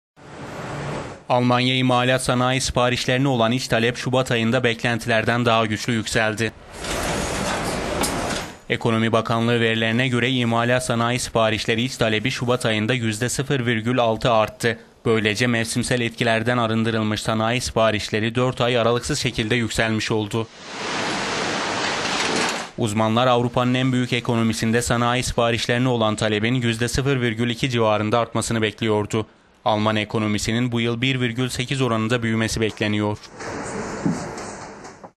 Almanya imalat sanayi iç talebinde güçlü artış - economy
Almanya imalat sanayi siparişlerine olan iç talep, şubat ayında beklentilerden daha güçlü yükseldi.

Ekonomi Bakanlığı verilerine göre, imalat sanayi siparişleri iç talebi şubat ayında yüzde 0,6 arttı. Böylece mevsimsel etkilerden arındırılmış  sanayi siparişleri 4 ay aralıksız şekilde yükselmiş oldu.

Uzmanlar Avrupa'nın en büyük ekonomisinde sanayi siparişlerine olan talebin yüzde 0,2 civarında artmasını bekliyordu.

Talep artışı ile beraber Almanya'da y...